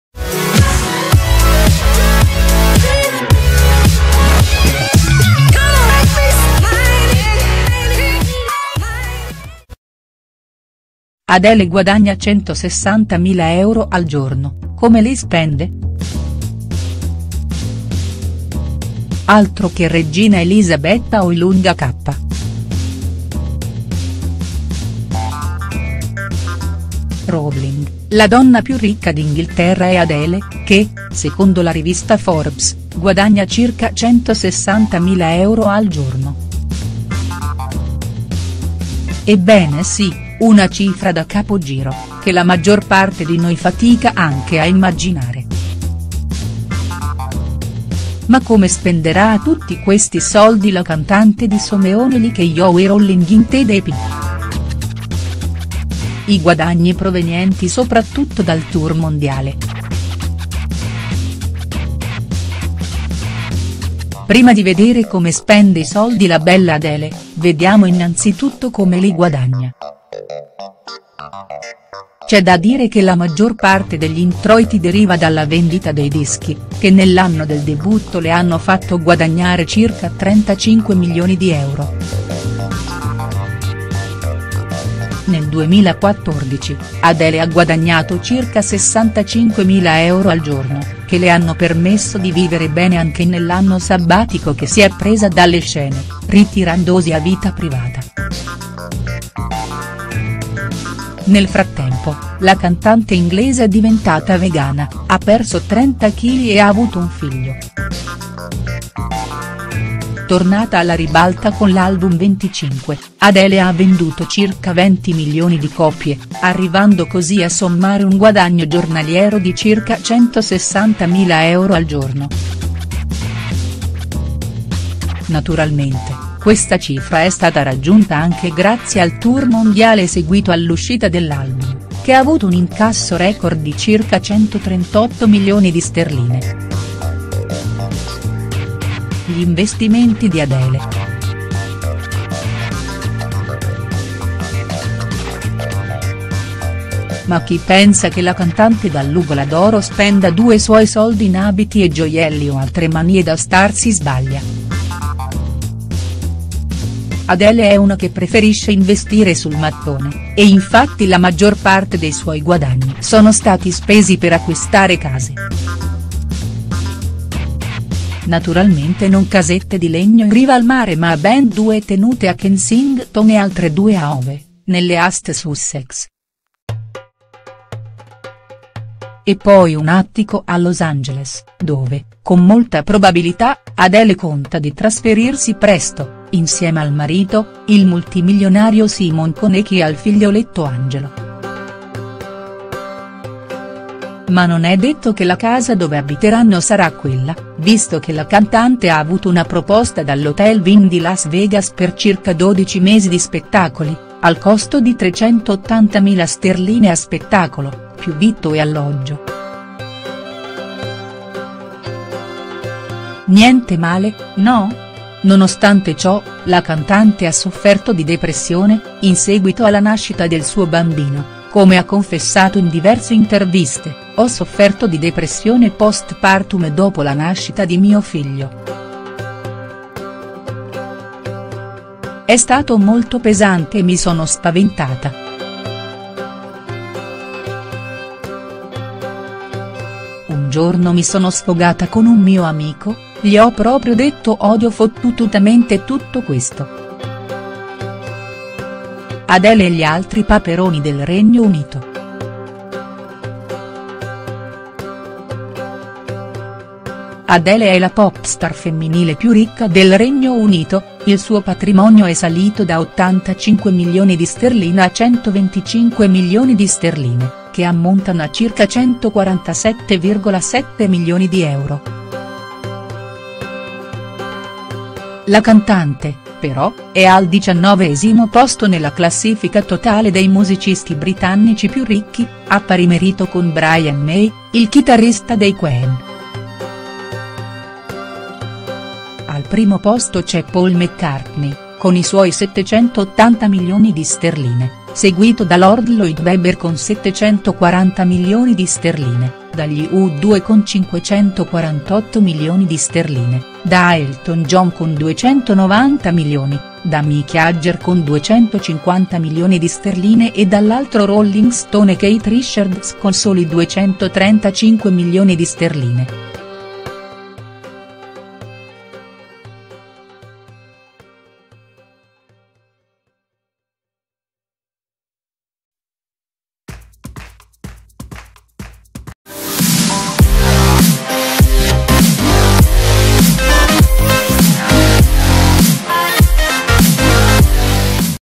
Ad... 160mila euro al giorno, come li spende?| STARS NEWS
Nonostante sia famosa per i suoi brani strappalacrime, densi di sofferenza, nella vita reale Adele ha ben poco da piangere, almeno a sentire Forbes. Ma la cantante più pagata del pianeta come spende i 160mila euro che guadagna ogni giorno?